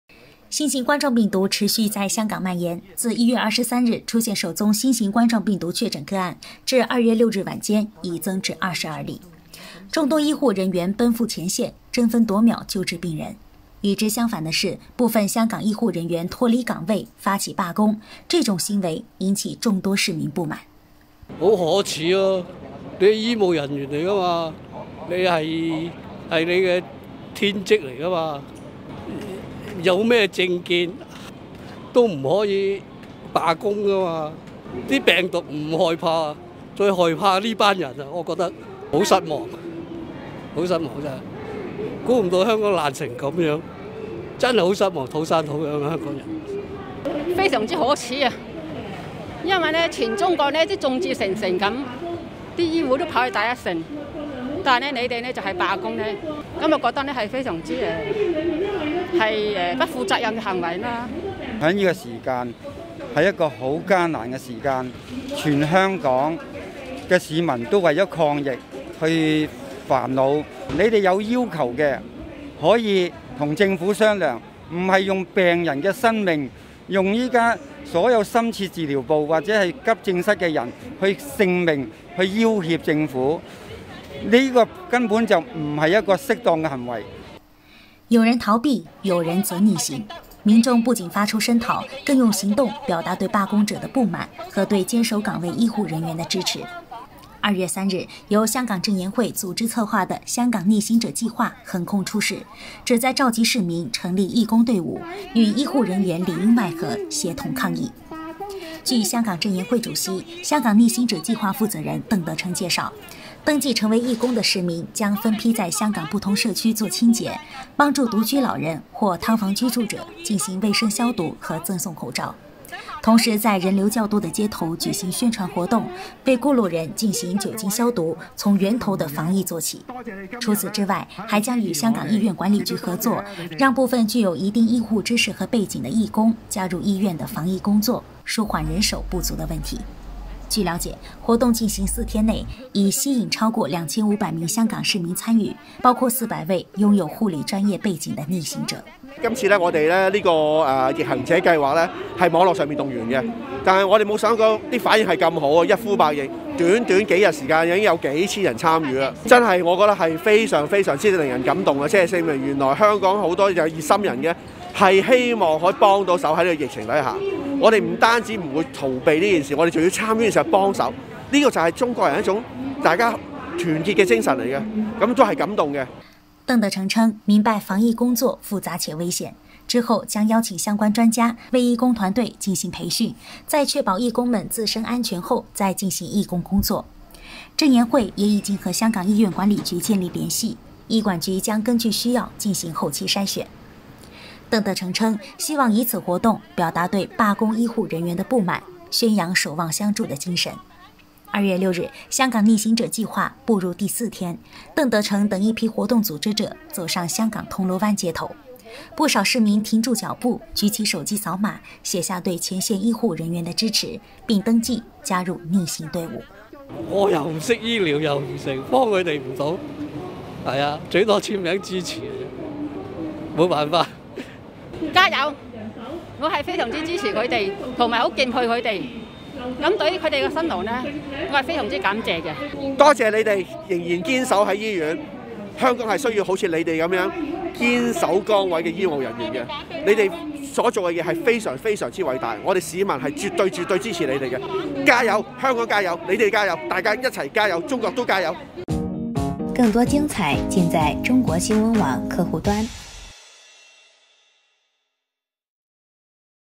香港市民自发组建“义工队”：你们因惧罢工，我们用爱“逆行”
2月6日，“香港逆行者”计划步入第四天，邓德成等一批活动组织者走上香港铜锣湾街头。新冠肺炎疫情下，香港众多医护人员奔赴前线；与之相反的是，部分香港医护人员脱离岗位，发起"罢工”，这种行为引起众多市民不满。有人逃避，有人则“逆行”，民众不仅发出声讨，更用行动表达对罢工者的不满和对坚守岗位医护人员的支持。